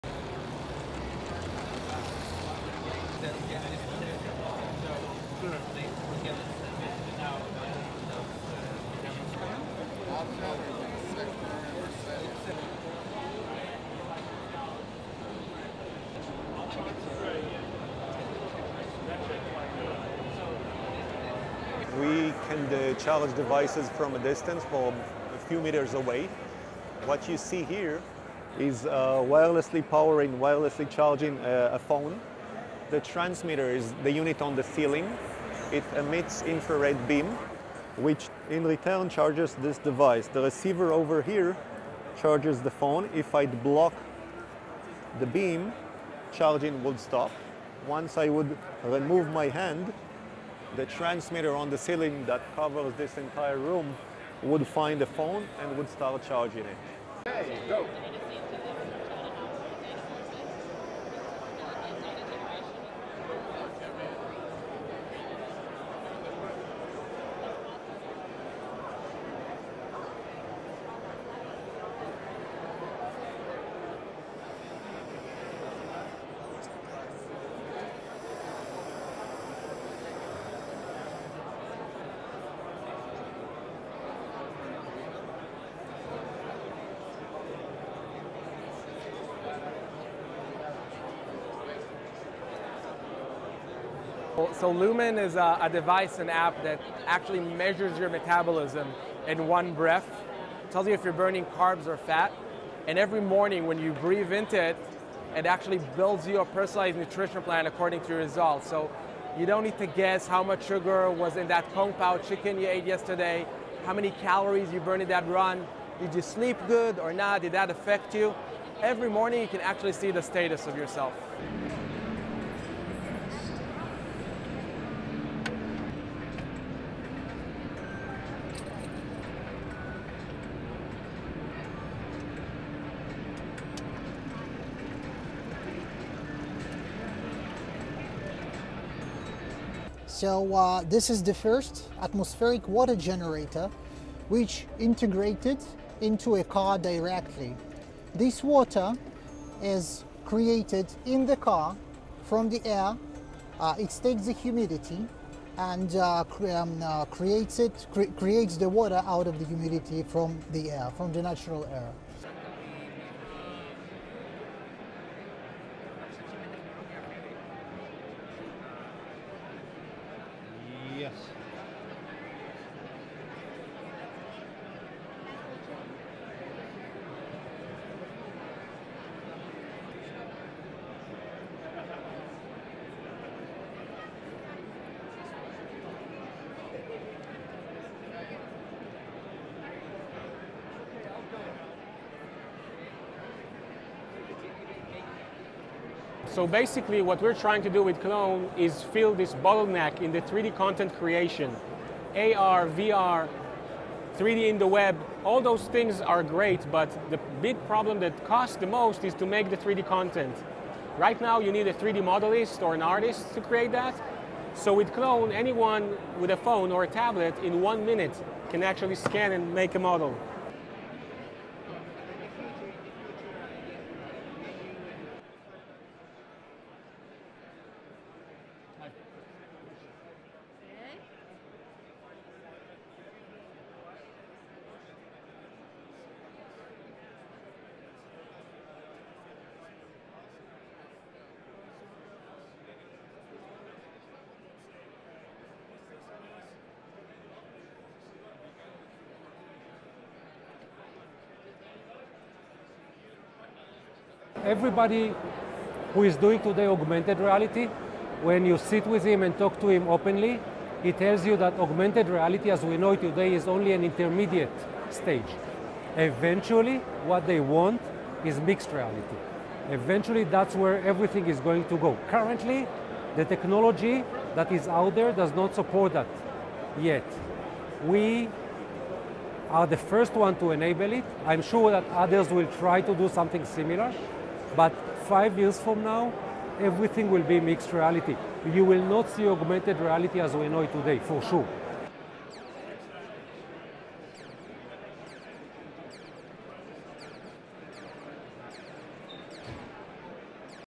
Israeli tech dominates at CES gadget show
(14 Jan 2019) US ISRAELI CES
SOURCE: ASSOCIATED PRESS
RESTRICTIONS: AP CLIENTS ONLY
LENGTH: 5.01

Las Vegas, US - 8 January 2019
1. Various of toy train powered solely by Wi-Charge's infrared wireless energy transfer kit
2. Close of sign reading (English) "This train has no batteries"
3. Wi-Charge light transmitter
4. Hand waving across phone fitted with a Wi-Charge receiver for wireless energy
5. SOUNDBITE (English) Ori Mor, co-founder and VP of research and development at Wi-Charge explains:
"We can charge devices from a distance of a few metres away. What you see here is wirelessly powering, wirelessly charging a phone. The transmitter is the unit on the ceiling. It emits infrared beam, which in return charges this device. The receiver over here charges the phone. If I block the beam, charging would stop. Once I would remove my hand, the transmitter on the ceiling that covers this entire room, would find the phone (and) would start charging it."
6. Various of Dror Ceder, founder of Lumen, using breath analyser device
7. Close of app showing the result of breath analysis
8. SOUNDBITE (English) Dror Ceder, founder of Lumen:
"So Lumen is a device, an app, that actually measures your metabolism in one breath. It tells you if you're burning carbs or fat. And every morning when you breathe into it, it actually builds your personalised nutrition plan according to your results.  So, you don't need to guess how much sugar was in that Kung Pao chicken you ate yesterday. How many calories you burnt in that run. Did you sleep good or not? Did that affect you?  Every morning you can actually see the status of yourself."
9. Various of Michael Rutman, Head of sales and marketing at Watergen, pouring glass of water in car, extracted from Watergen device
10. SOUNDBITE (English) Michael Rutman, Head of sales and marketing at Watergen:
"So this is the first atmospheric water generator which integrates into a car directly. This water is created in the car from the air, it takes the humidity, and creates the water out of the humidity from the natural air."
11. Various of man scanning toy horse using the Qlone app
12. Various of phone showing horse animation
13. SOUNDBITE (English), Ronen Horovitz, CEO of Eye Cue:
"So basically what we're trying to do with Qlone is fill this bottleneck in the 3D content creation. AR, VR, 3D in the web - all those things are great -  but the big problem that costs the most is to make the 3D content. Right now you need a 3D modelist or an artist to create that. So, with Qlone, anyone with a phone or a tablet in one minute can actually scan and make a model."
14. Close of horse animation
15. Various of mixed reality app from 2SENS. Figure interacts in real time with the real world
16. SOUNDBITE (English) Moshe Meyassed, CEO of 2SENS:
"Everybody who is doing today augmented reality, when you sit with him and talk to him openly, he tells you that augmented reality as we know it today is only an intermediate stage. Eventually what they want is mixed reality. Eventually that's where everything is going to go. Currently, the technology that is out there does not support that, yet. We are the first ones to enable it. I'm sure that others will try to do something similar. But five years from now everything will be mixed reality. You will not see augmented reality as we know it today. For sure."
17. Various of mixed reality app from 2SENS
LEADIN
Israeli tech has been flexing its muscle at the Customer Electronics Show in Las Vegas.
SCRIPT
Ori Mor, co-founder and VP of research and development at Wi-Charge explains: